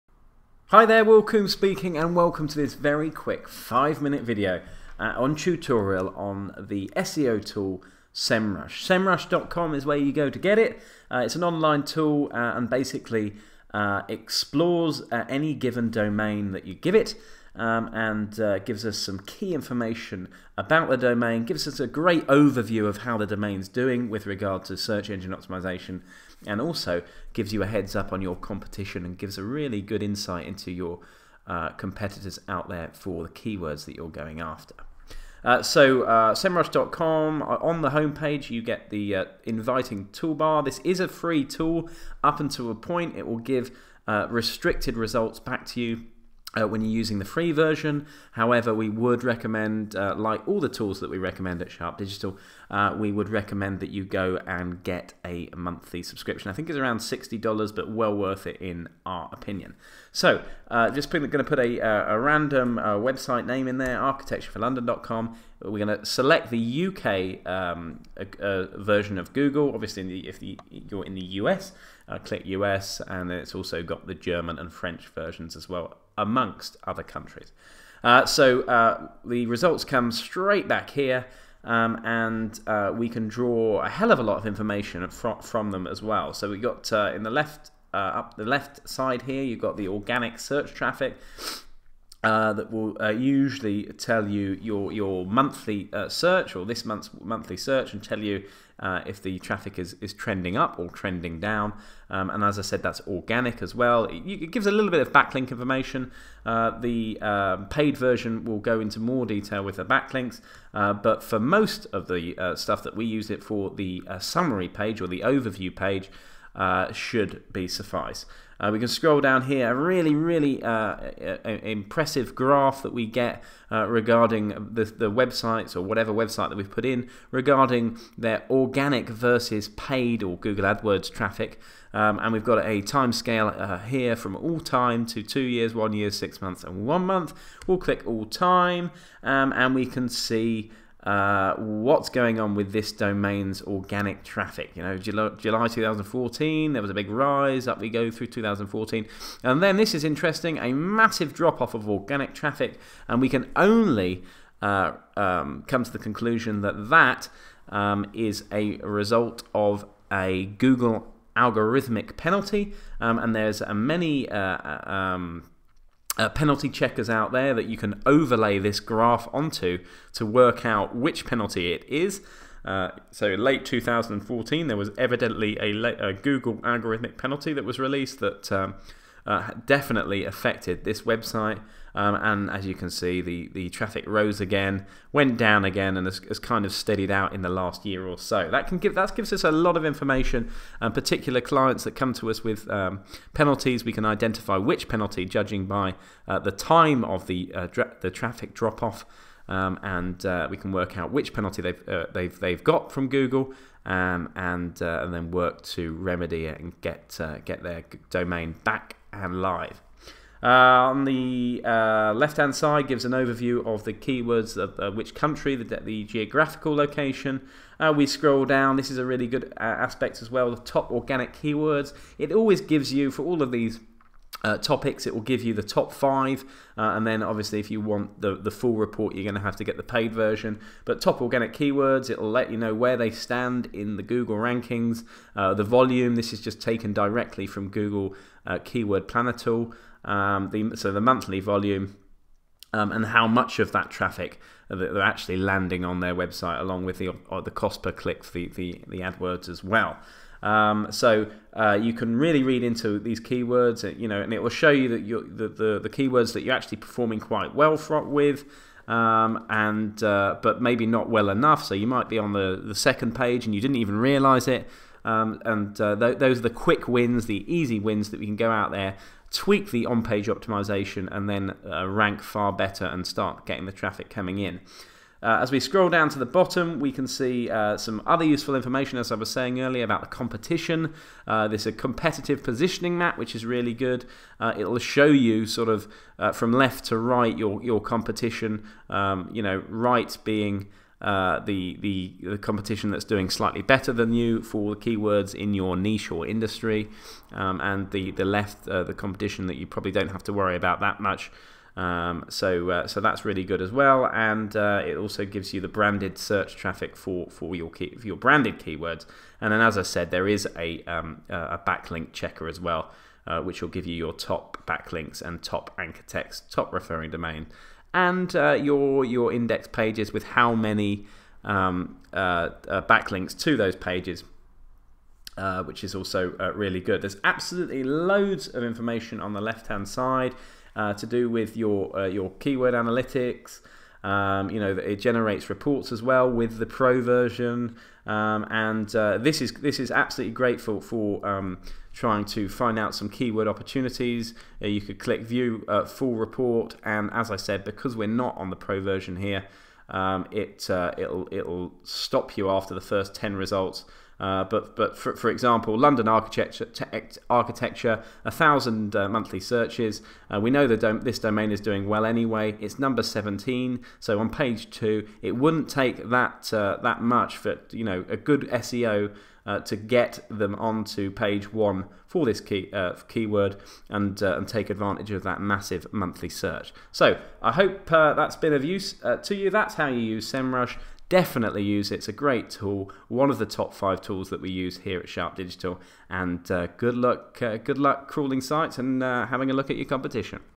SEO | Using SEMrush
The 5-minute "all you need to know" tutorial on SEMrush. A superfast SEO analysis & competition analysis